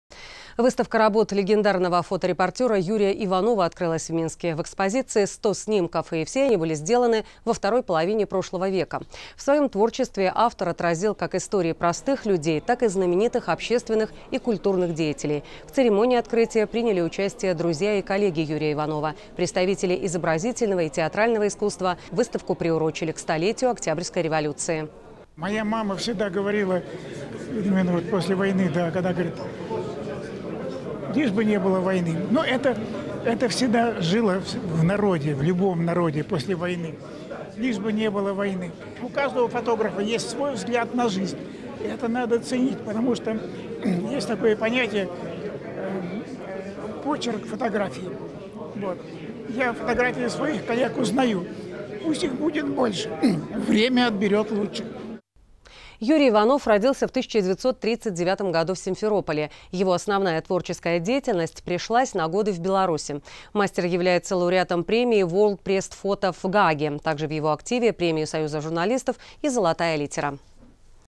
​Выставка работ легендарного фоторепортёра Юрия Иванова открылась в Минске
В экспозиции – 100 снимков, все они были сделаны во второй половине прошлого века.